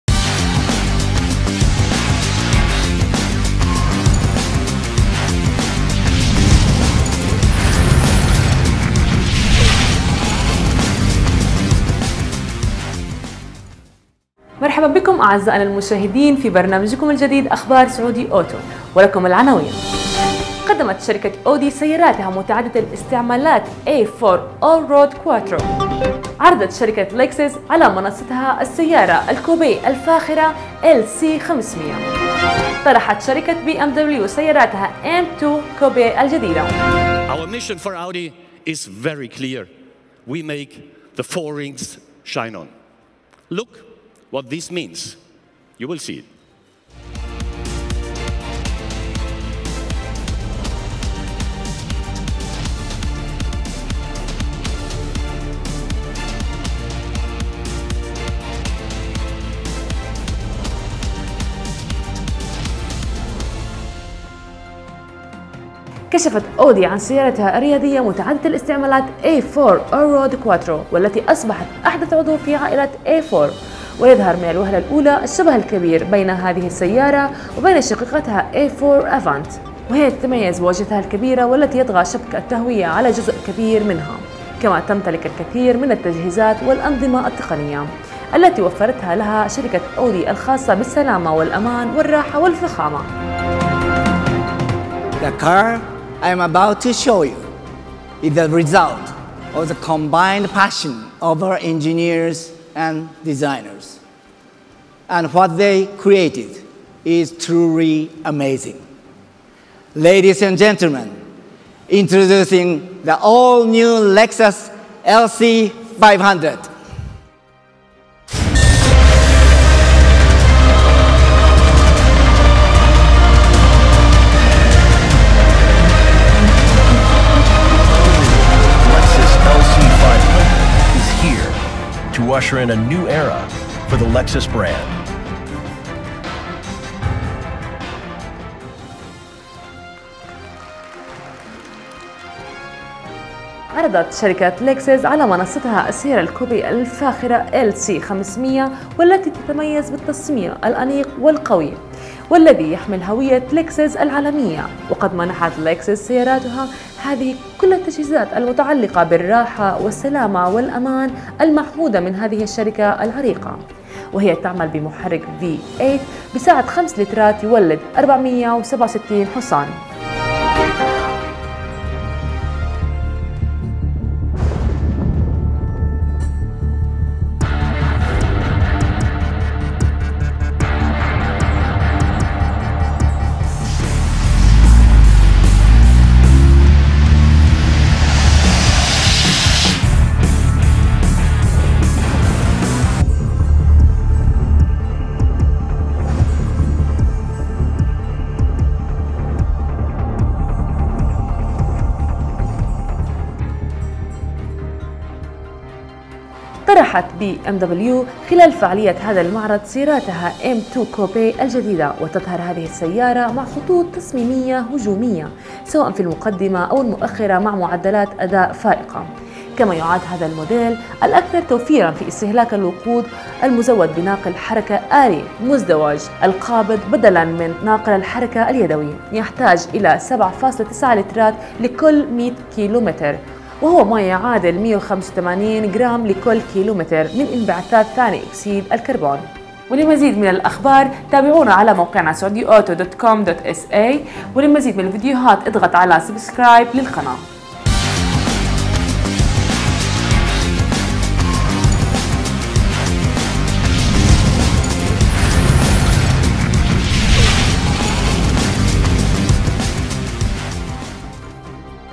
أخبار سعودي أوتو - معرض ديترويت - NAIAS 2016 - الحلقة الثانية
أخبار سعودي أوتو برنامج جديد نتمنى أن يحقق مزيد من الفائدة لكم و أن ينال إعجابكم .
وهذه الحلقة الثانية التي ننقل لكم من خلالها الجزء الأول من أهم و أبرز الأحداث ضمن فعاليات معرض شمال أمريكا (ديترويت) لسنة 2016